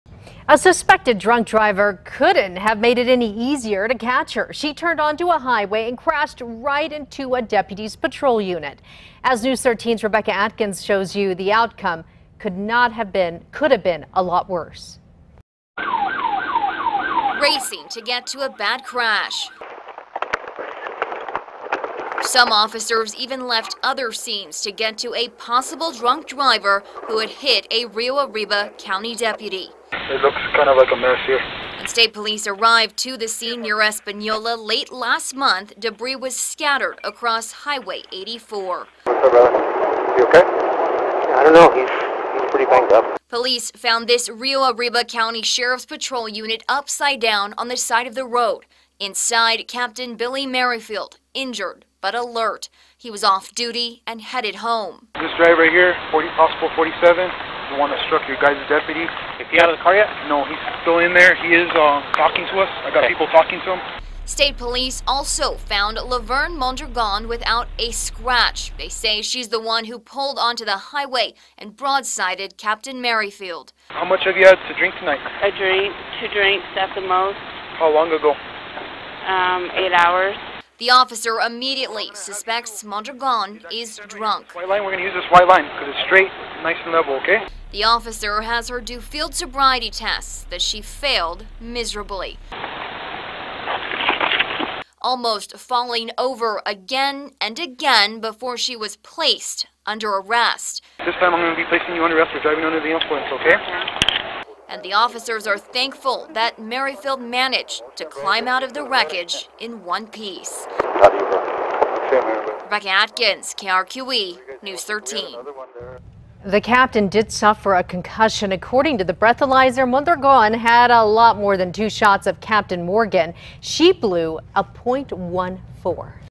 Police video shows moments after deputy hit by alleged drunk driver
Police video shows moments after deputy hit by alleged drunk driver - Source